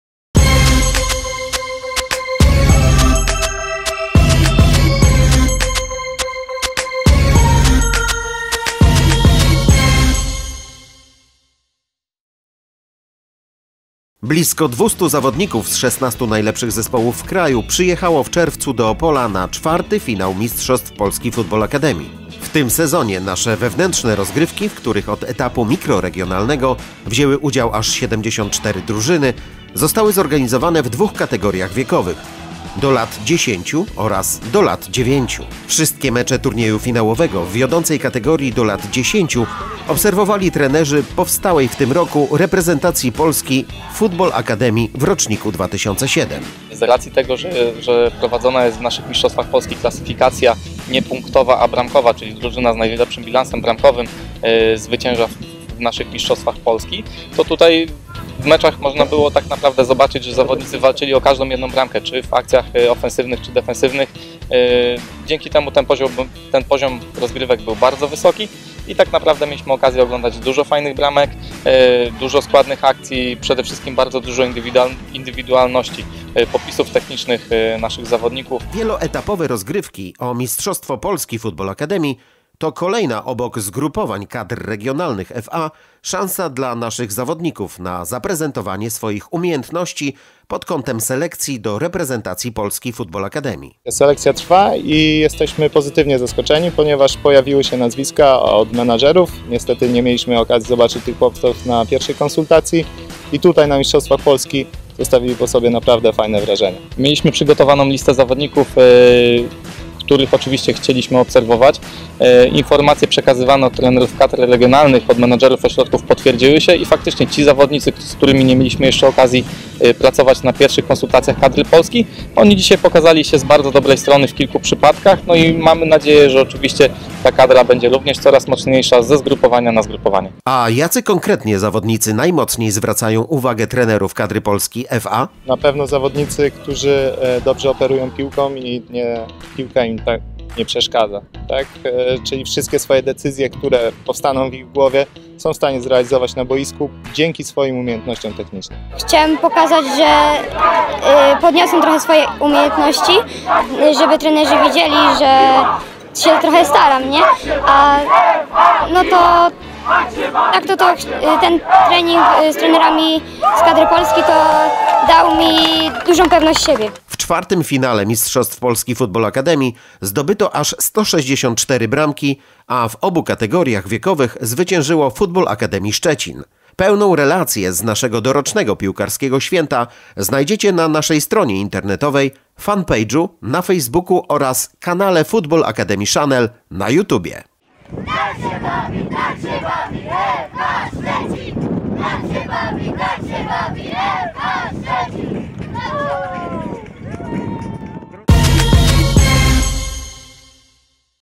Finał Mistrzostw Polski FA 2017
Wieloetapowe rozgrywki o Mistrzostwo Polski Football Academy to doskonała okazja do zaprezentowania swoich umiejętności trenerom Reprezentacji Polski FA!